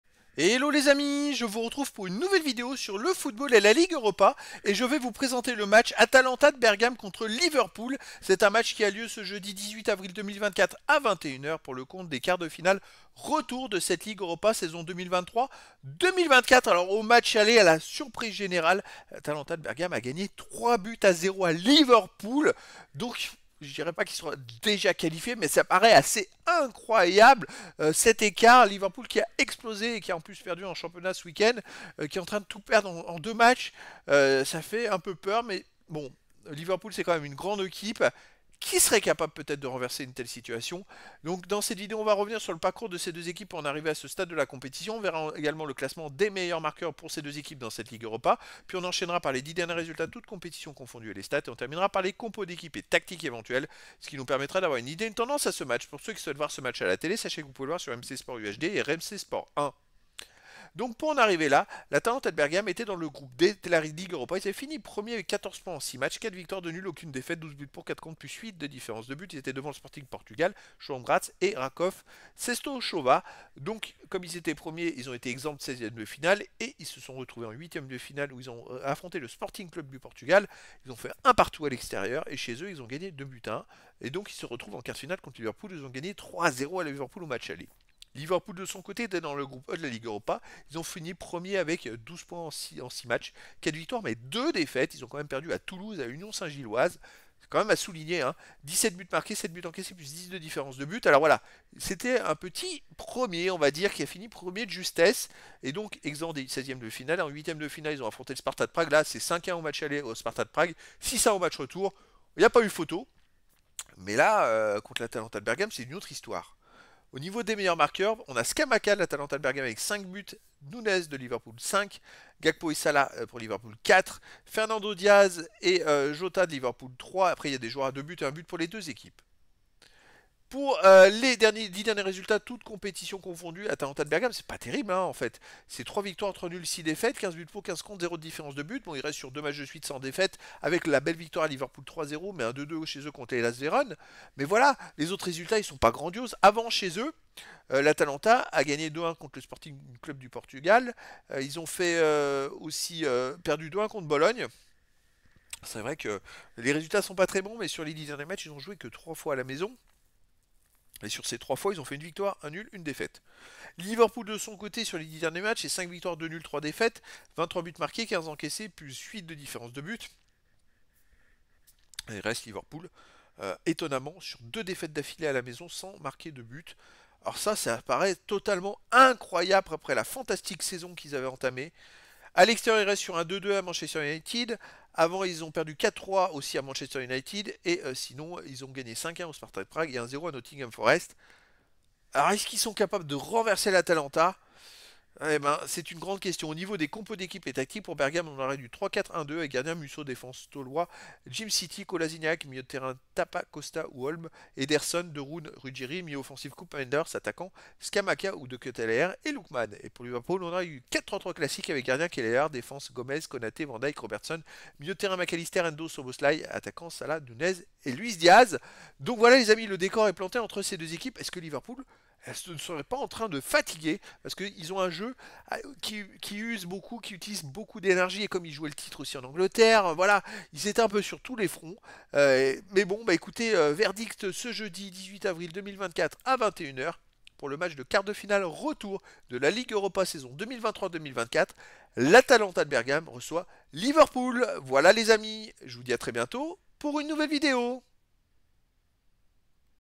ATALANTA BERGAME - LIVERPOOL : Quart de Finale Retour - Ligue Europa Saison 2023/2024
Je vous présente le match de football de Quart de Finale Retour de la Ligue Europa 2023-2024, match du 18/04/2024 : Atalanta Bergame contre Liverpool.

Au match aller l'Atalanta Bergame a gagné 3 buts à 0 à Liverpool.

Je vous présente le parcours de ces 2 équipes pour en arriver à ce stade de la compétition avec les stats, le classement des buteurs des joueurs des 2 équipes dans la ligue Europa, les résultats des 2 équipes sur les 10 derniers matchs toutes compétitions confondues avec les stats, les compositions éventuelles des 2 équipes avec les tactiques et stratégies.